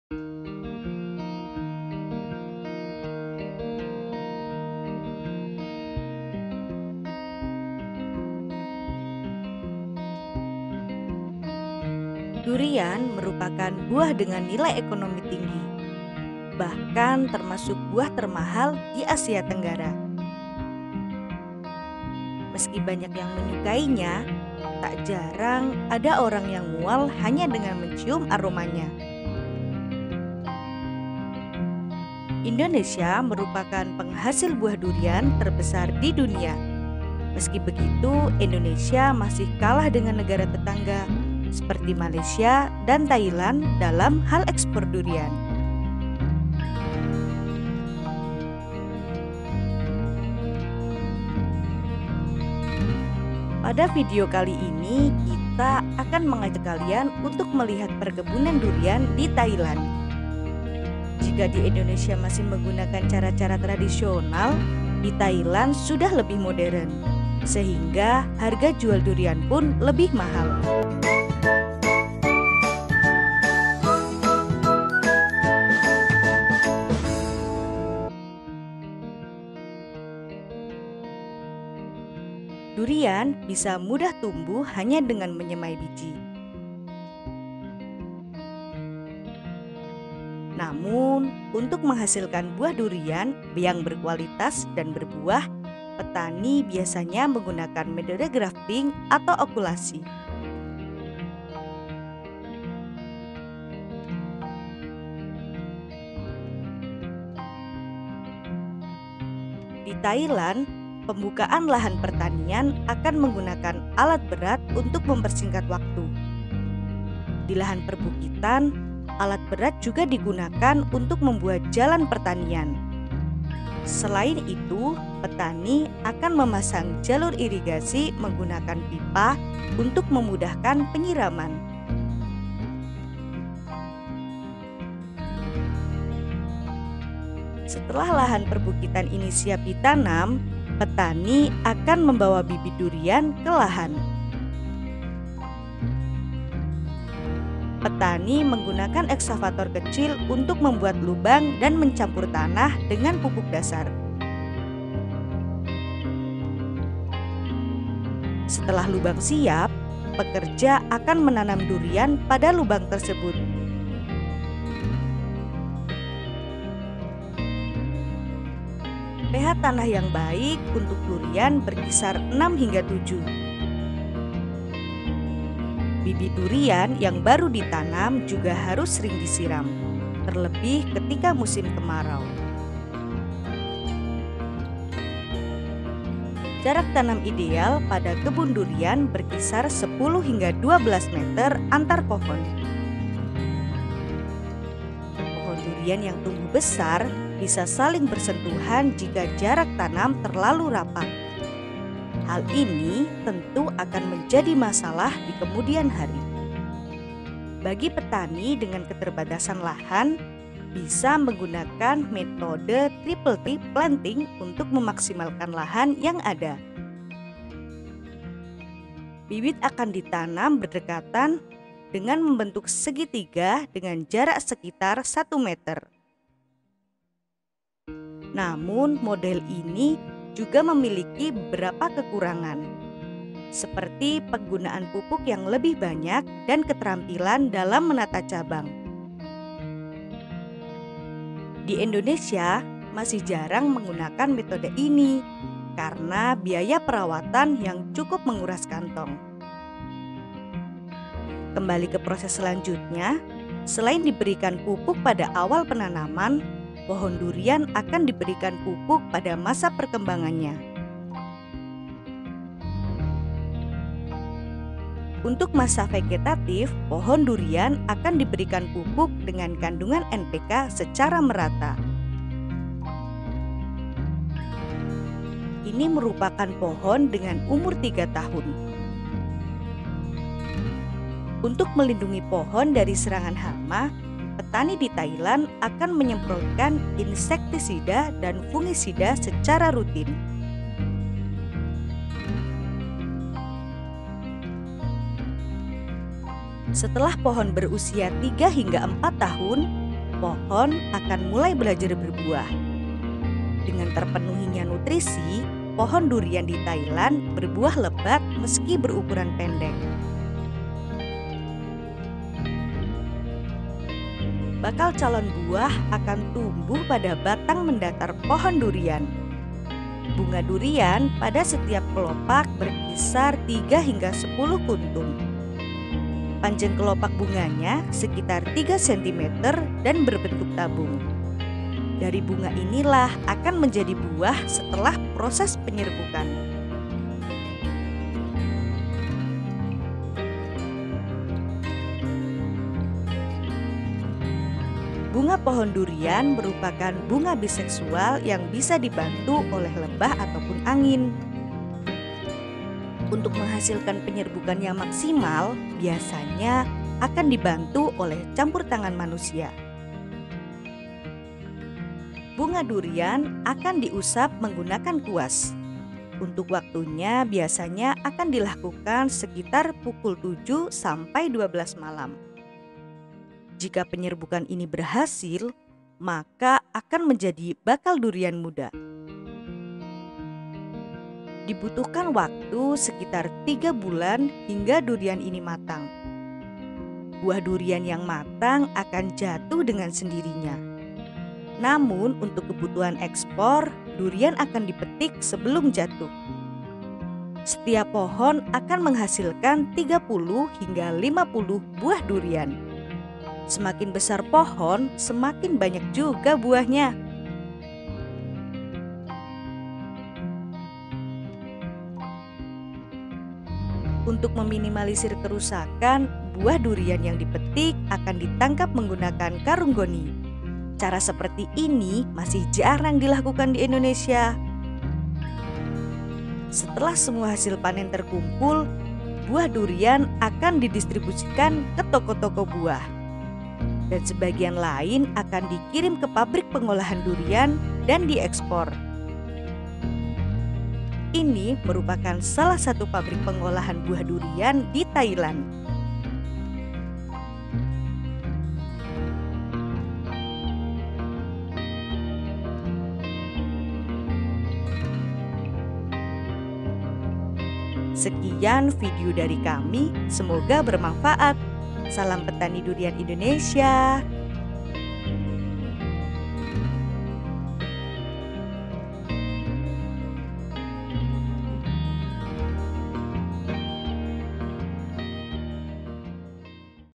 Perbedaan Kebun Durian Thailand dan Indonesia, Pantesan Indonesia Kalah !
Indonesia merupakan negara penghasil buah durian terbesar di dunia. meski begitu, indonesia masih kalah dalam hal pertanian durian dibanding Thailand. seperti apa sih kebun durian di thailand ? simak video berikut sampai selesai ya

Untuk informasi seputar dunia pertanian bisa kalian baca di